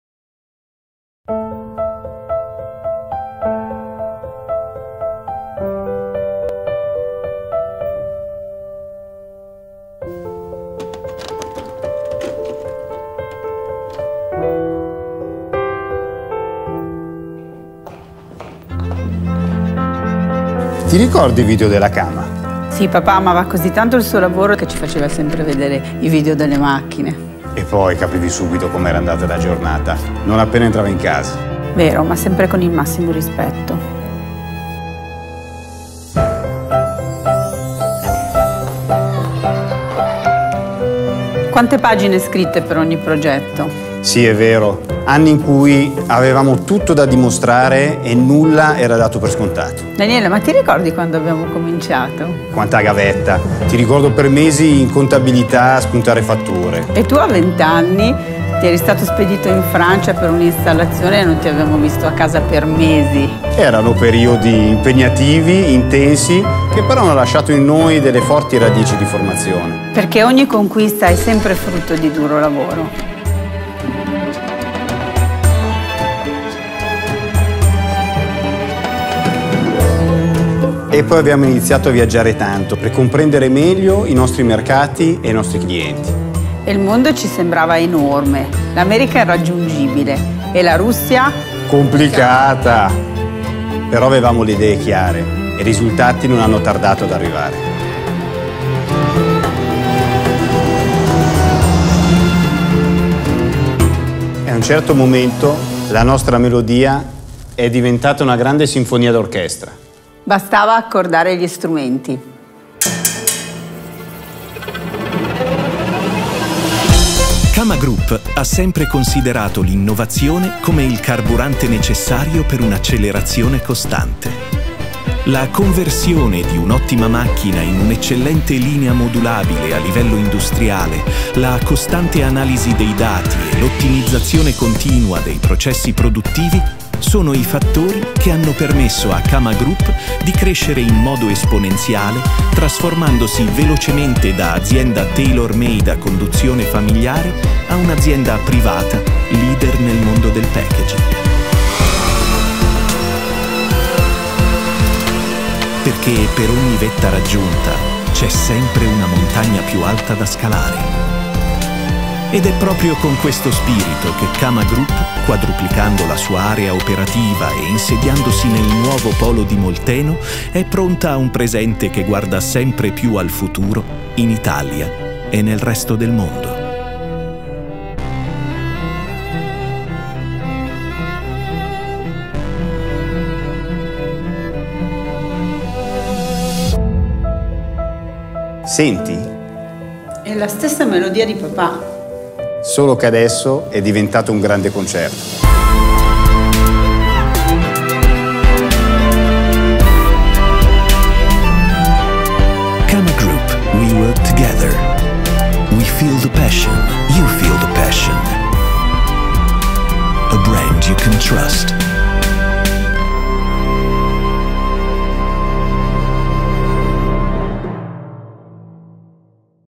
I primi 40 anni di Cama Group
Quest’anno è un anno molto speciale per noi! Abbiamo sicuramente fatto molta strada da quando è stata installata nel 1981 la  prima macchina Cama.
Esprimiamo la nostra gratitudine a tutti i clienti, dipendenti, agenti e fornitori che hanno contribuito al nostro continuo successo.